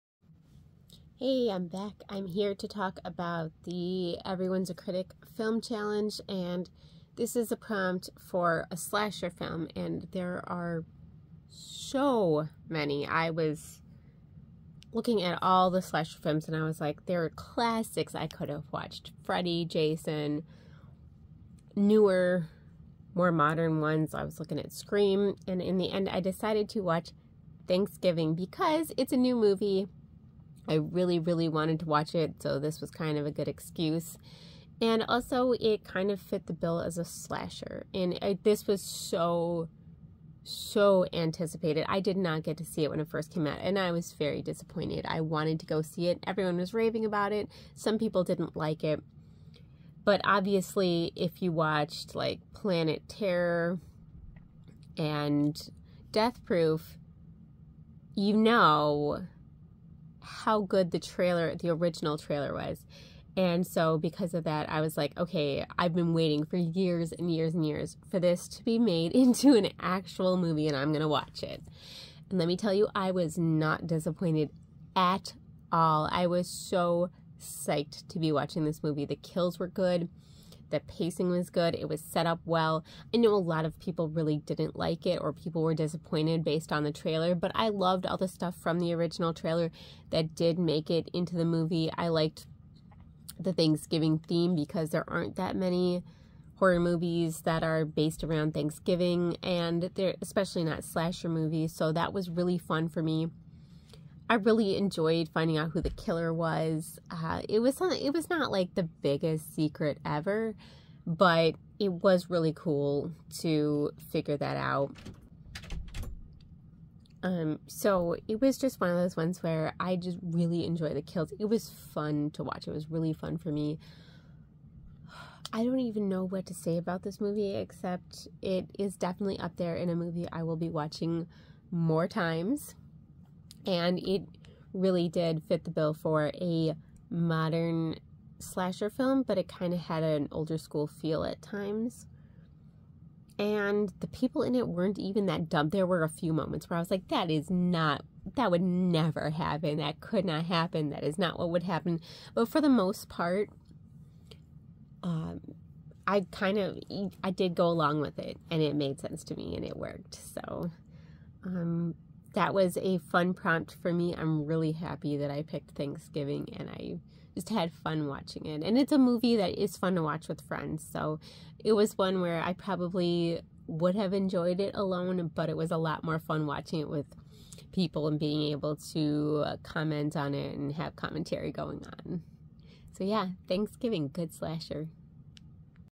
Movie Talk - Thanksgiving (2023)
With spoilers
For the Everyone's A Critic horror movie challenge prompt for a slasher film.